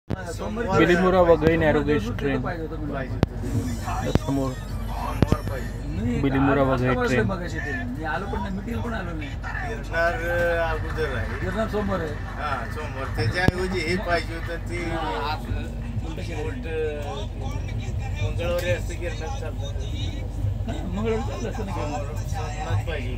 09501 Bilimora Waghai Narrow Gauge Train spotted at Bilimora Junction.
09501 Bilimora Waghai Narrow Gauge Train spotted at Bilimora Junction.